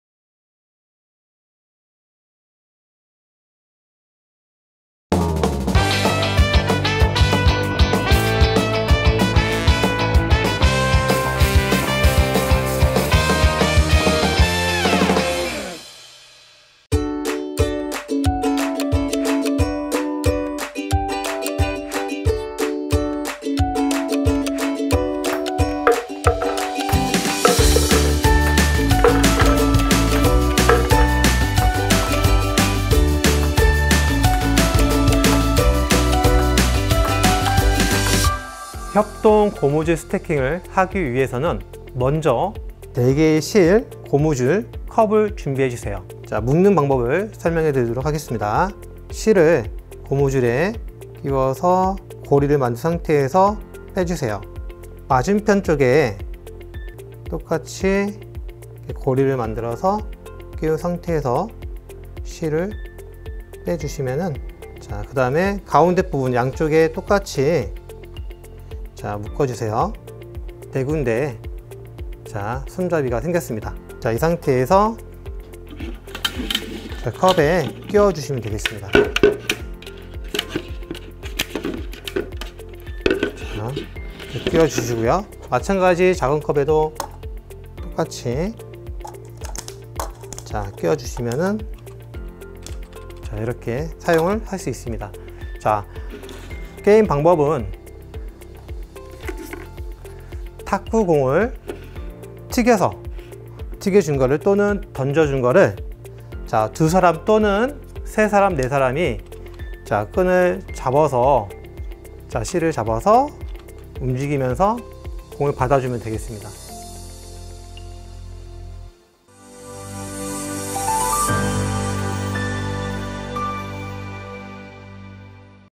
Cooperative rubber band Cup Stacking| Icebreaker | Team-building Game | Made in Korea
✅ Materials Needed
1️⃣ cheetha cups
2️⃣rubber bands
3️⃣string

- Stacking Korea [Activity Cup] Try it like this

🤗Education consultation/order of supplies